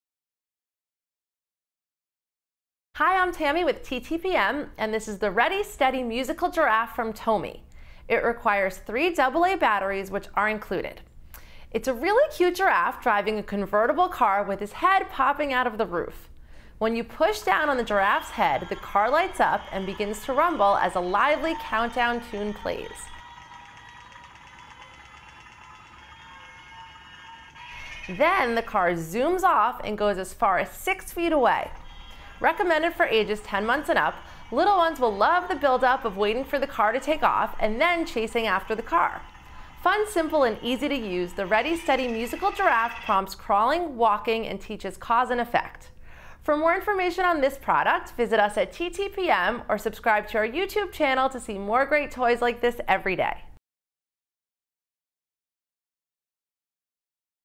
Ready Steady Musical Giraffe from TOMY
When you push down on the Ready Steady Musical Giraffe's head, the toy car lights up and begins to rumble as a countdown tune plays. Then the car zooms off and goes as far as six feet away. For full review and shopping info

Product Info:
The Ready Steady Musical Giraffe toy features a cute giraffe driving a convertible car with his head popping out of the roof. When you push down on the giraffe's head, the car lights up and begins to rumble as a lively, countdown tune plays. Then the car zooms off and goes as far as six feet away.